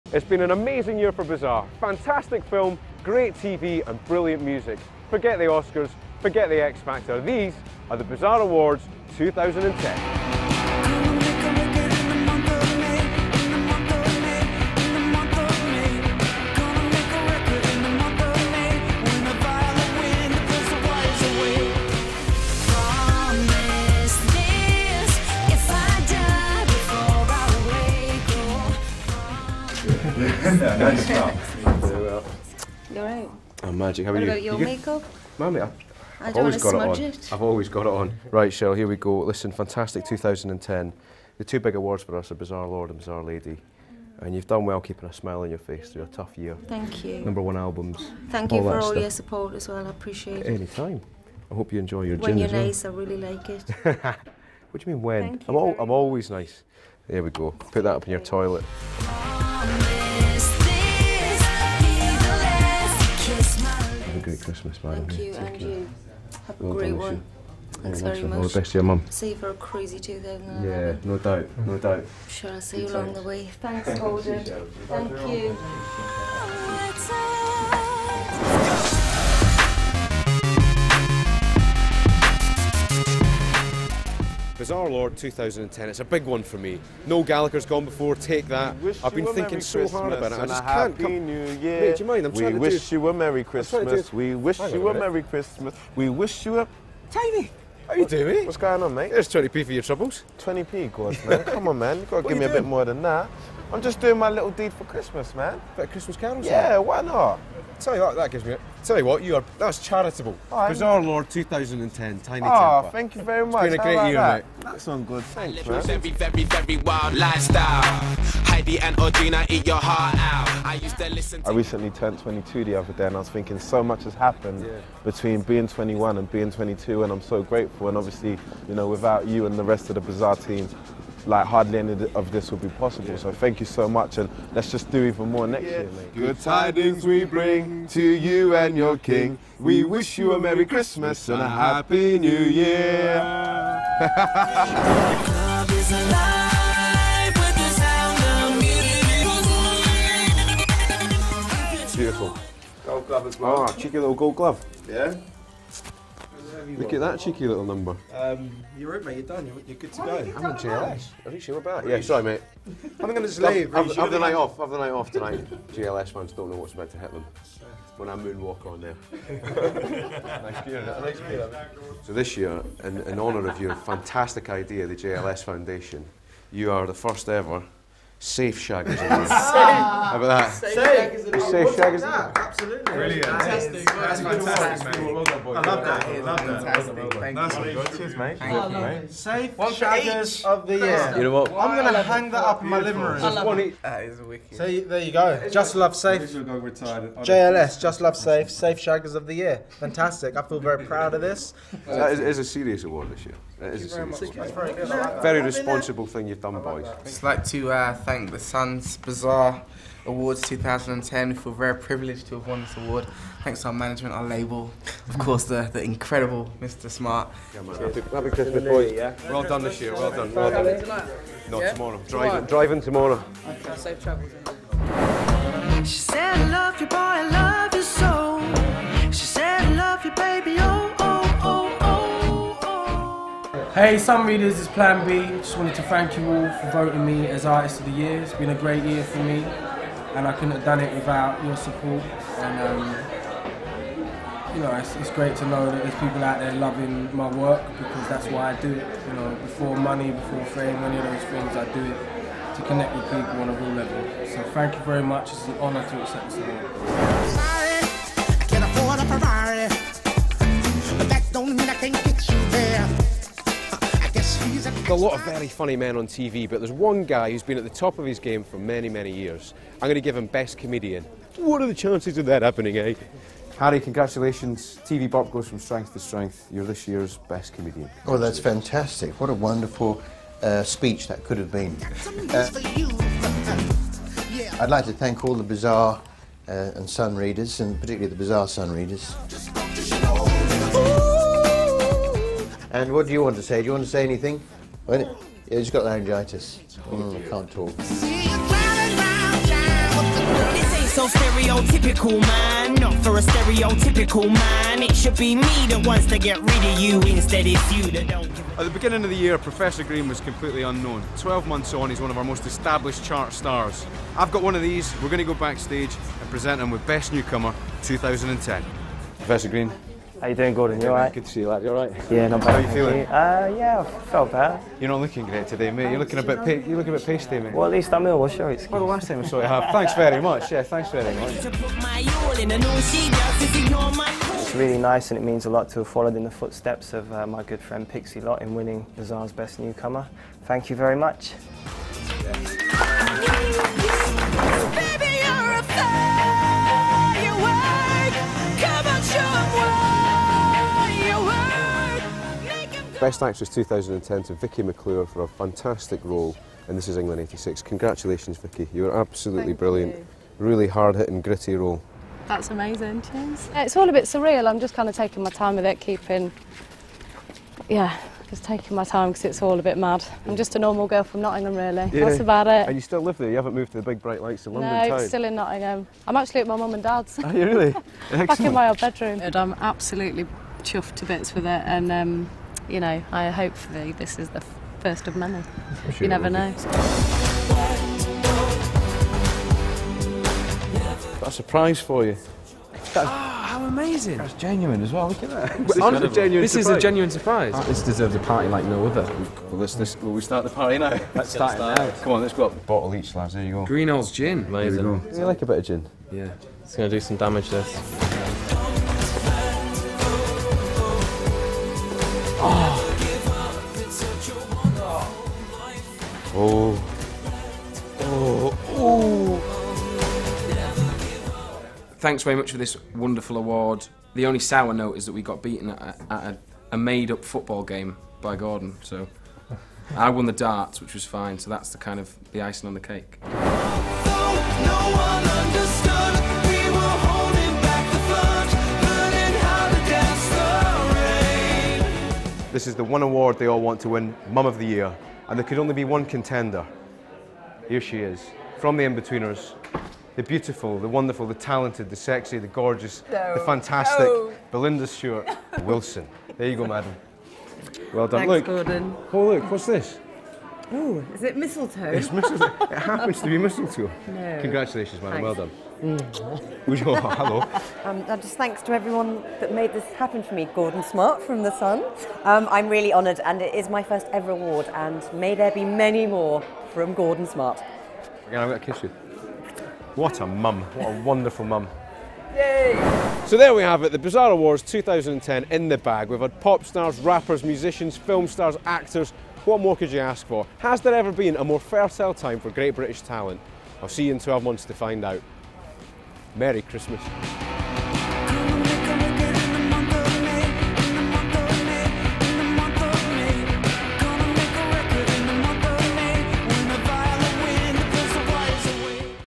GREENALL'S GIN & THE SUN BIZARRE AWARDS 2010
Britain's favourite gin, Greenall's and Britain's best loved tabloid, The Sun, celebrated the best of British talent with the 2010 Sun Bizarre Awards. Check out the awards handed to stars including Cheryl Cole, Tinie Tempah, JLS and Professor Green.